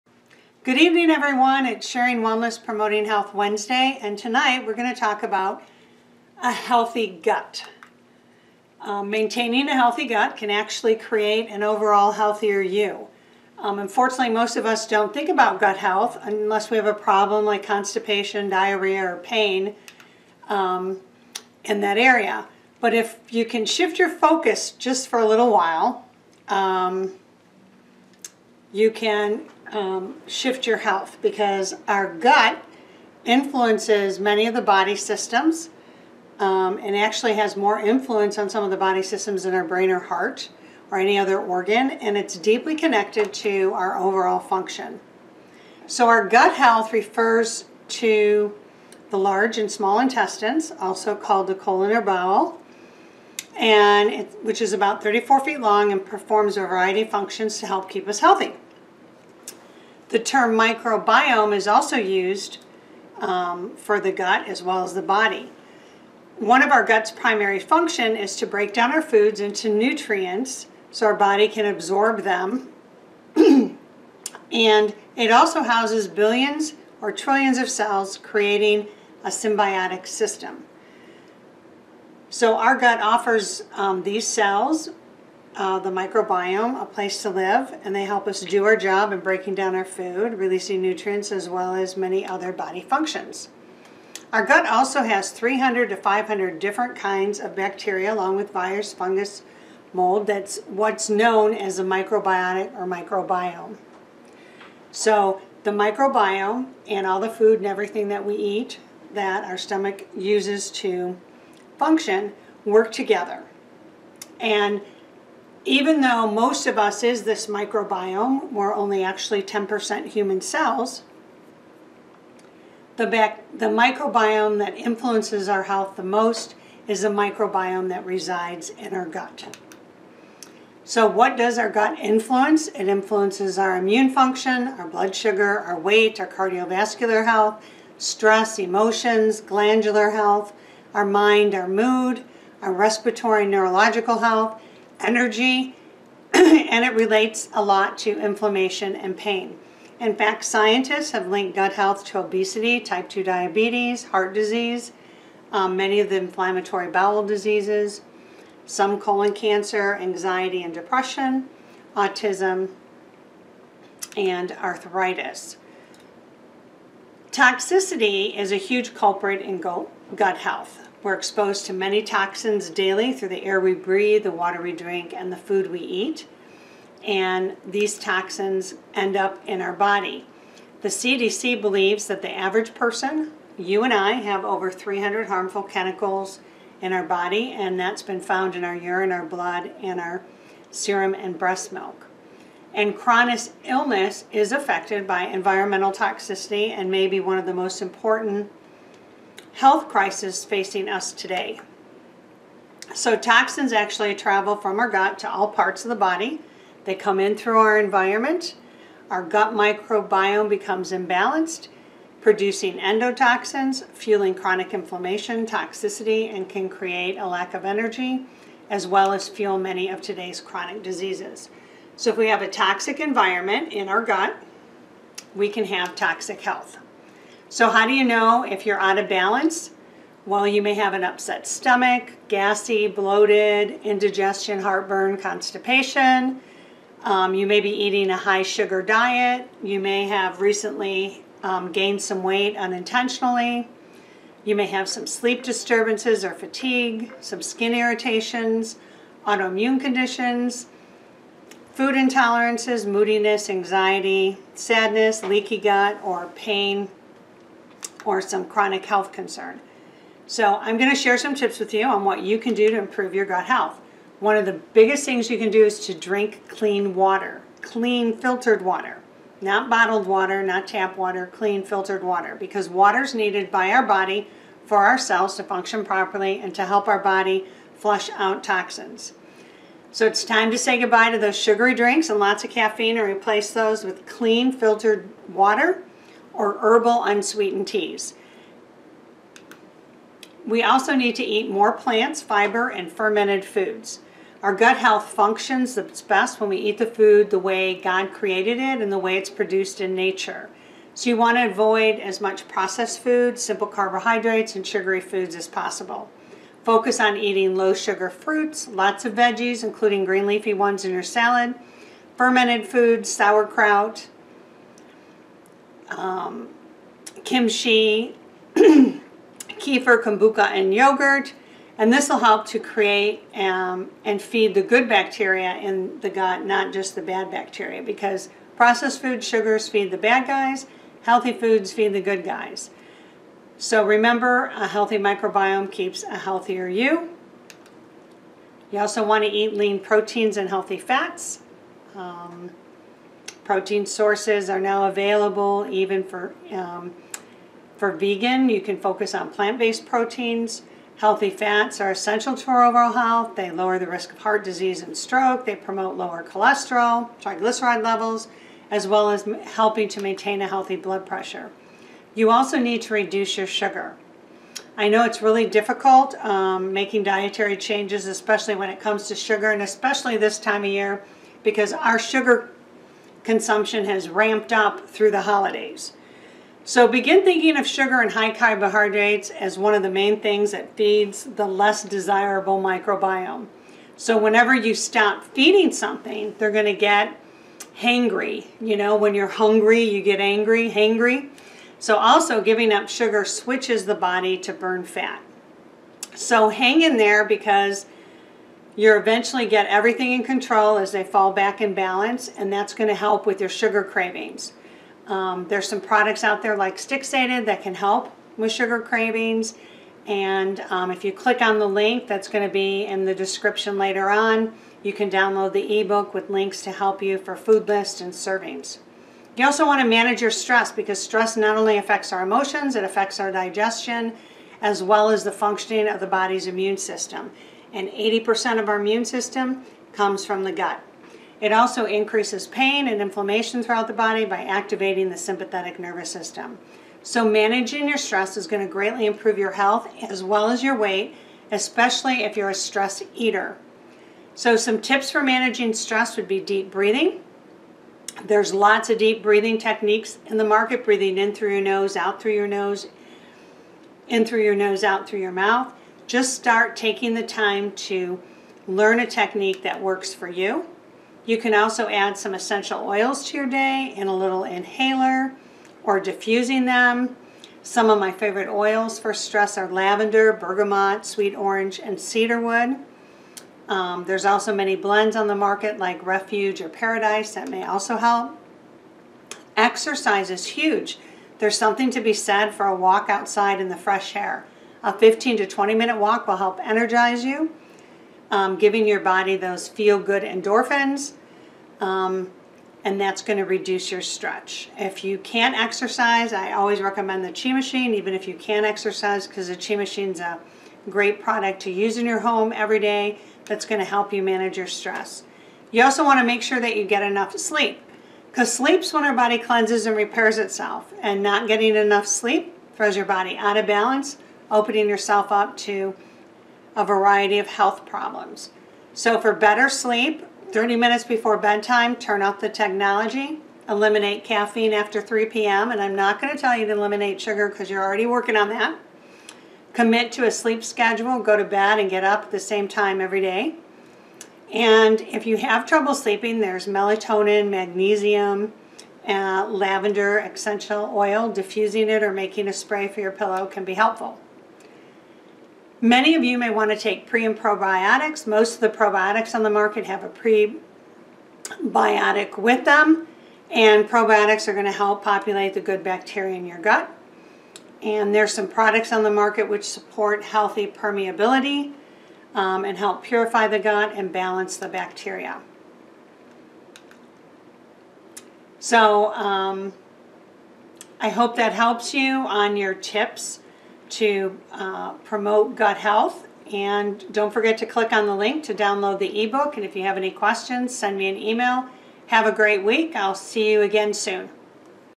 Gut Health Impacts your Overall Health | Keep your Gut Healthy
✤ Welcome to Sharing Wellness Promoting Health Wednesday with Energy Wellness Products ✤

Many people don't realize that gut health dramatically impacts how we feel. It interacts with all the systems in the body and can impact the immune system, pain, mood, weight, and more. Learn how to improve your gut health and improve your feelings.

⏰ Video Duration: 00:09:44
YouTube Chapters:
0:00 - Introduction
2:10 - What Does Our Gut Influence
7:24 - Lean Proteins and Healthy Fats
7:59 - Exercise
⌚ SCHEDULE:
⌚ New Video very Other Week
👉 Thank you for supporting “ Energy Wellness Products ”.